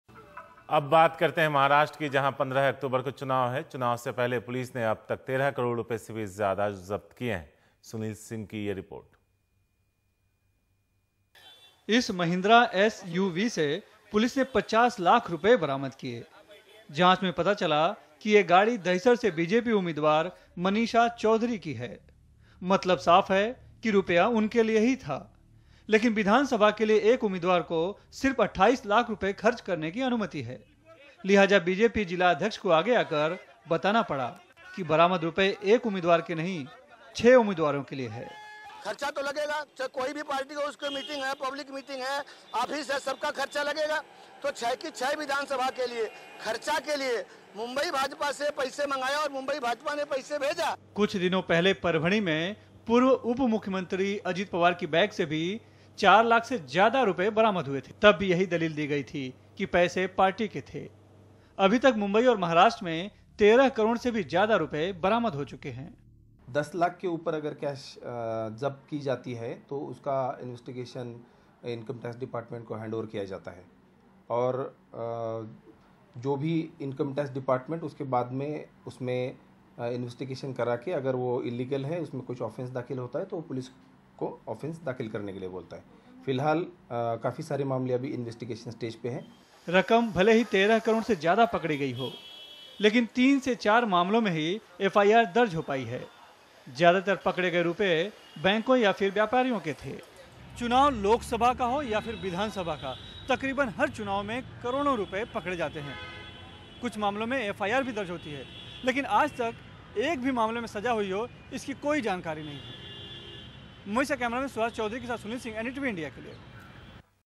Over 13 crore rupees seized in raids in Maharashtra
Cash exceeding 13 crore rupees have been seized during raids conducted by the Maharashtra police before polls to check on corrupt practices during elections. According to the police, each time cash exceeding 10 lakh rupees is found in the possession of anybody, the matter gets transferred to the IT department for investigating the source of those funds. (Audio in Hindi)